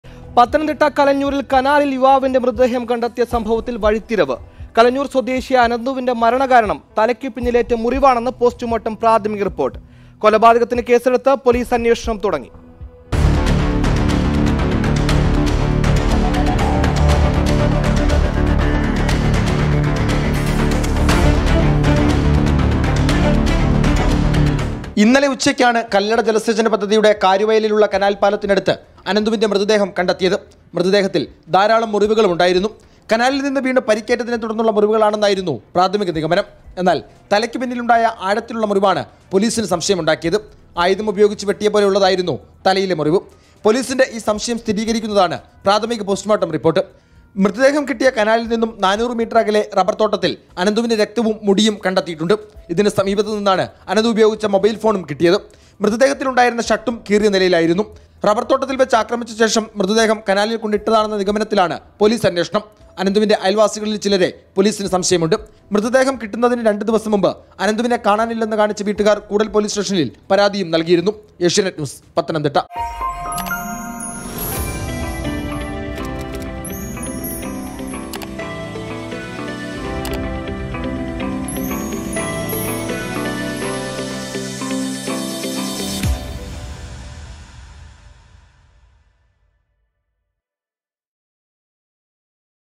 കനാലിൽ യുവാവിന്റെ മൃതദേഹം കണ്ടെത്തിയ സംഭവത്തിൽ വഴിത്തിരിവ്
തലയിൽ ആയുധം ഉപയോ​ഗിച്ച് വെട്ടിയപോലുള്ള മുറിവ്; മൃതദേഹം കണ്ടെത്തിയ കനാലിന് സമീപമുള്ള റബർതോട്ടത്തിൽ അനന്തുവിന്റെ രക്തവും മുടിയും കണ്ടെത്തി, അയൽവാസികളെ കേന്ദ്രീകരിച്ച് പോലീസ് അന്വേഷണം
Deadbody in canal | Pathanamthitta